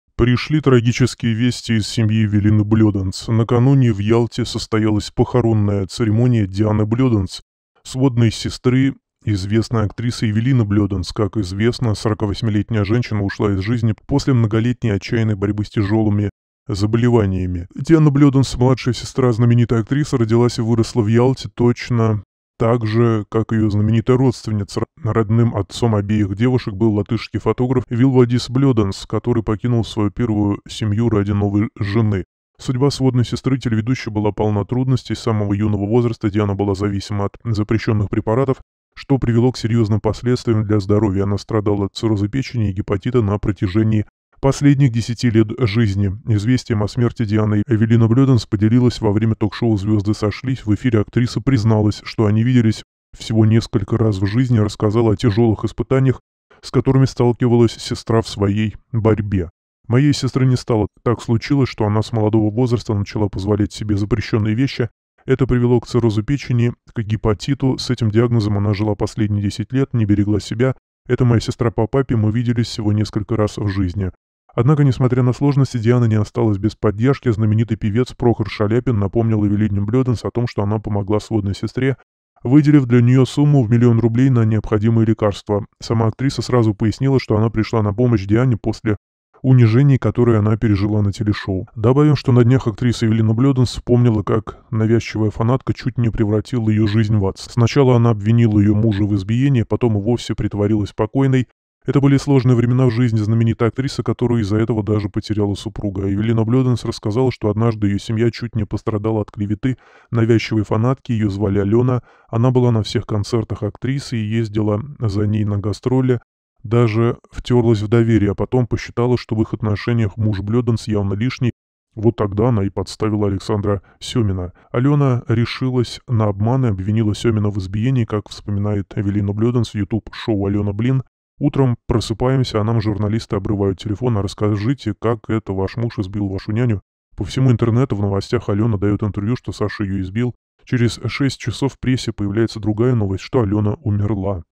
Пришли трагические вести из семьи Эвелины Блёданс
Накануне в Ялте состоялась похоронная церемония Дианы Блёданс, сводной сестры известной актрисы Эвелины Блёданс. Как стало известно, 48-летняя женщина ушла из жизни после многолетней и отчаянной борьбы с тяжелыми болезнями.